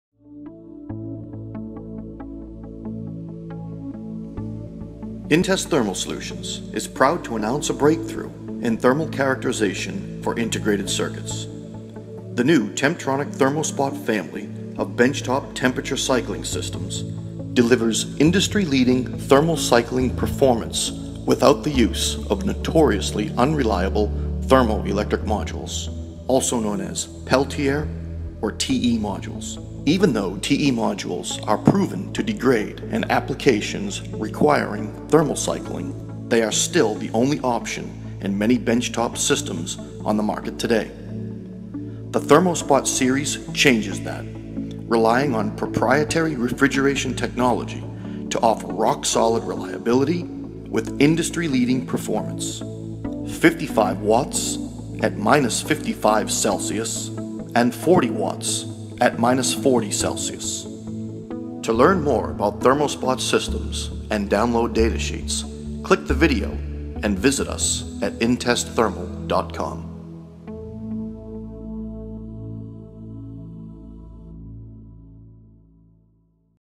IC Thermal Characterization System
inTEST Thermal Solutions is proud to announce a breakthrough in thermal characterization for integrated circuits. The new Temptronic ThermoSpot family of benchtop temperature cycling systems delivers industry leading thermal cycling performance without the use of notoriously unreliable thermoelectric modules, also known as Peltier or TE modules.

Even though TE Modules are proven to degrade in applications requiring thermal cycling, they are still the only option in many benchtop systems on the market today. The ThermoSpot Series changes that, relying on proprietary refrigeration technology to offer rock-solid reliability.  (use GRAPHIC, No TE Modules) with industry leading performance    – 55w at -55C and-40W at -40C.